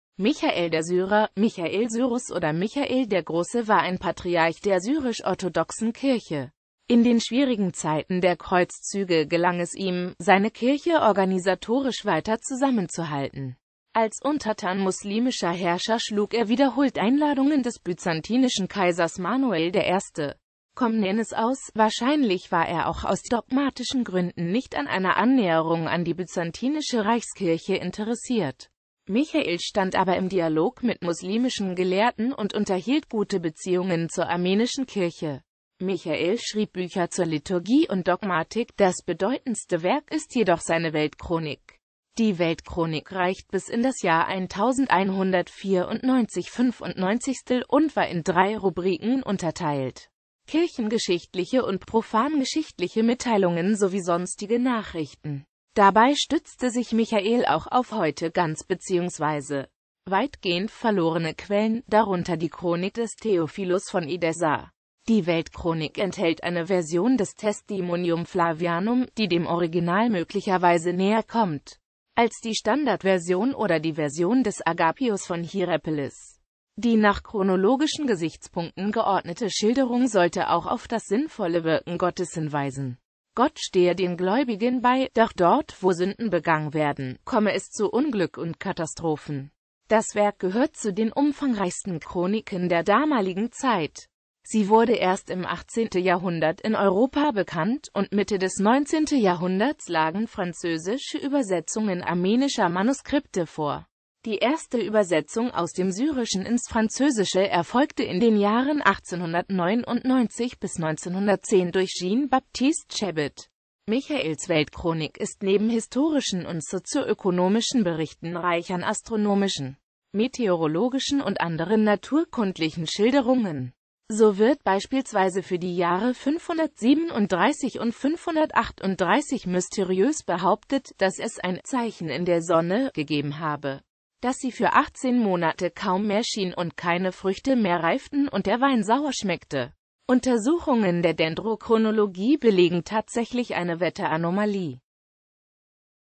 Michael der Syrer
Vielen Dank für Ihre Unterstützung:

Michael der Syrer, Michael Syrus oder Michael der Große war ein Patriarch der Syrisch-Orthodoxen Kirche.In den schwierigen Zeiten der Kreuzzüge gelang es ihm, seine Kirche organisatorisch weiter zusammenzuhalten.Als Untertan muslimischer Herrscher schlug er wiederholt Einladungen des byzantinischen Kaisers Manuel I.Komnenos aus; wahrscheinlich war er auch aus dogmatischen Gründen nicht an einer Annäherung an die byzantinische Reichskirche interessiert.